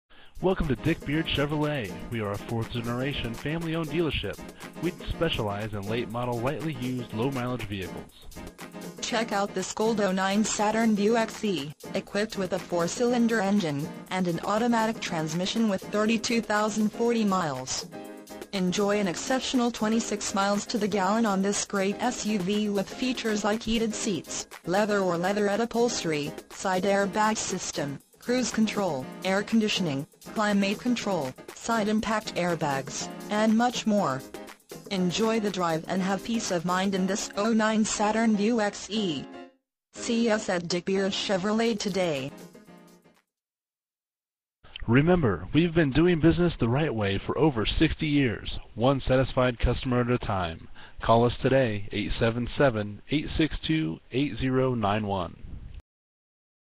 2009 Saturn Vue XE Dick Beard Chevrolet Hyannis, MA 02601
2009 Saturn Vue XE- Imagine driving this Gold 2009 Saturn Vue XE, equipped with a 4 Cyl. engine and an automatic transmission with 32,040 miles. Enjoy an exceptional 26 miles to the gallon on this great SUV with features like Heated Seats, Leather or Leatherette Upholstery, Side Air Bag System, Cruise Control, Air Conditioning, Climate Control, Side Impact Airbags, and much more. Enjoy the drive and have peace of mind in this 2009 Saturn Vue XE. See us at Dick Beard Chevrolet today!
Miles-  32,040
VIN- 3GSCL33PX9S607208

Dick Beard Chevrolet
22 Ridgewood Ave.